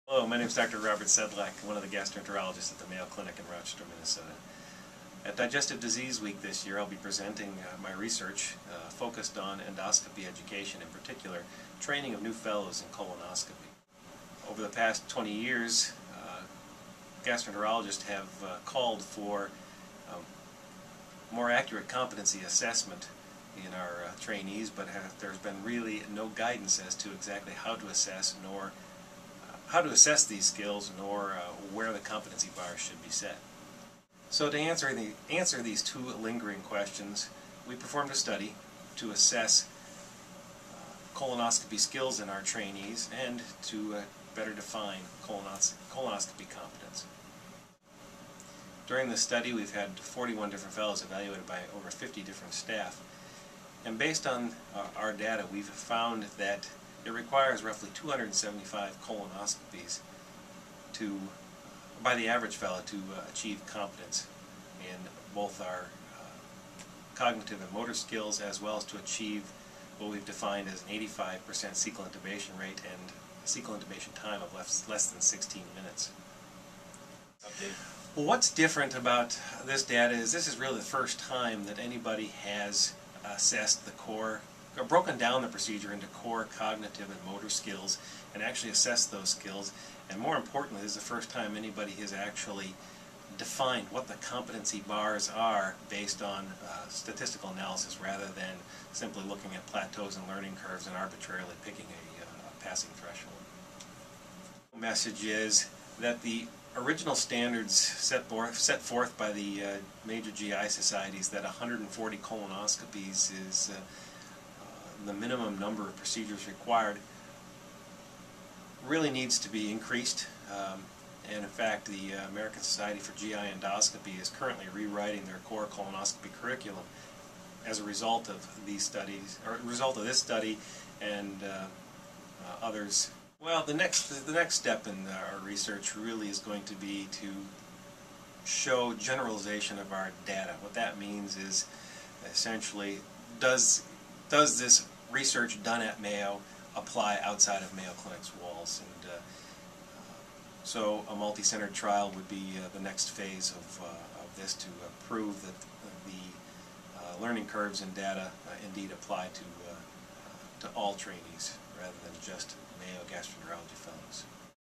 Mayo Clinic Studies How Much Practice Makes Perfect When Performing Colonoscopies
A colonoscopy is an invaluable procedure for detecting problems in the colon and rectum. Doctors can often diagnose gastrointestinal issues and even catch the warning signs of colorectal cancer. Perfecting the skills required for this delicate procedure takes practice. But just how much practice makes perfect?

That was the question Robert E. Sedlack, M.D., and his Mayo Clinic research team set out to answer in their recently completed study of colorectal procedures. Their findings suggest much more practice is needed than gastroenterological professional societies currently recommend.